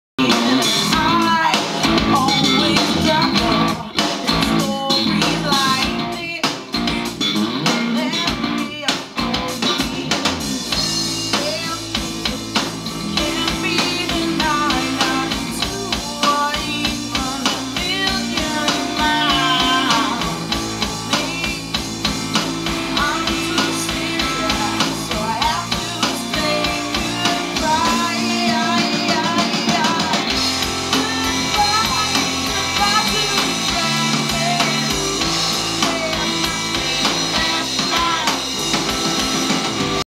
Sandberg California PM (studio session at the Bamboo room Cincinnati)
studio session with my Sandberg at the Bamboo Room , one of the best studo recording in Cincinnati.